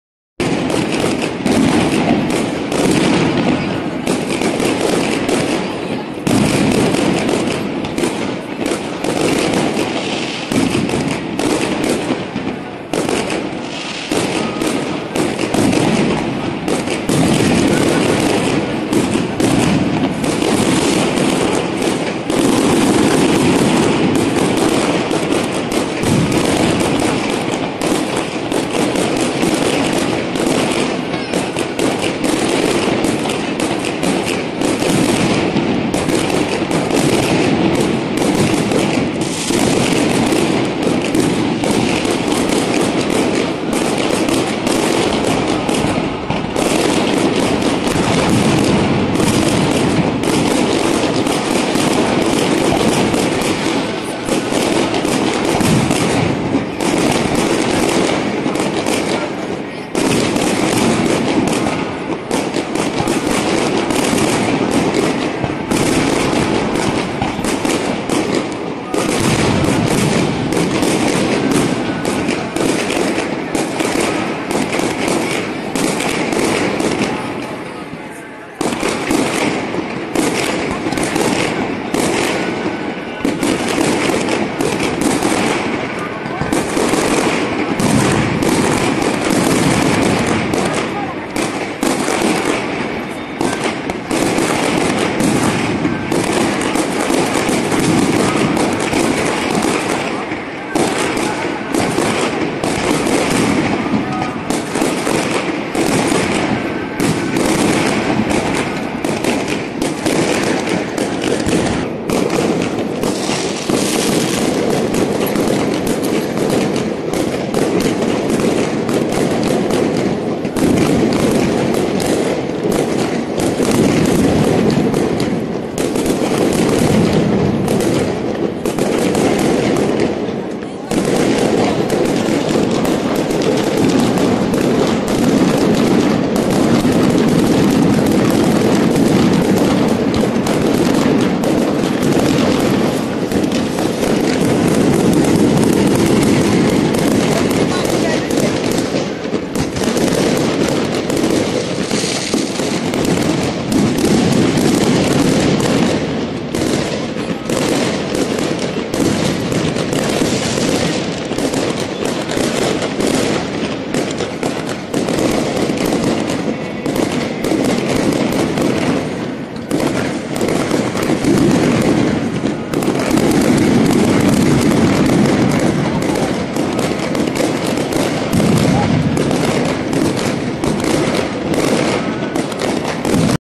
Beautiful Live Fire Crackers on Guru Gobind Singh ji Gurpurab
553 Parkash Purab of Guru Gobind Singh Ji